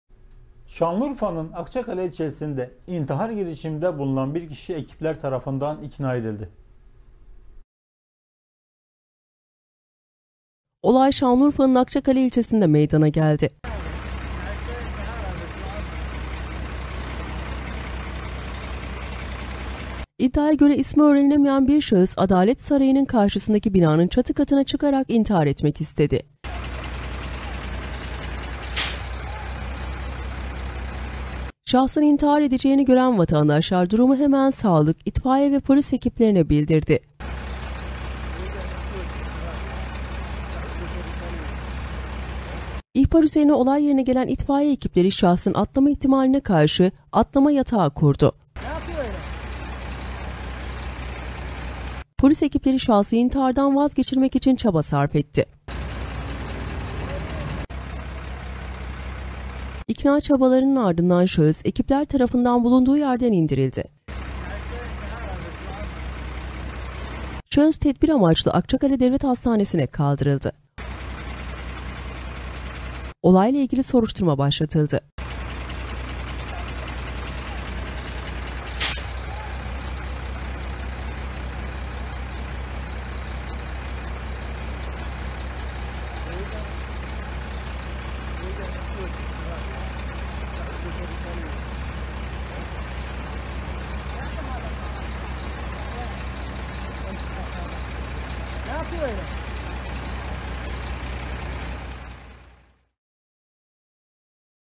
Akçakale'de İntihar Girişimi
Şanlıurfa’nın Akçakale ilçesinde intihar girişiminde bulunan bir kişi ekipler tarafından ikna edildi.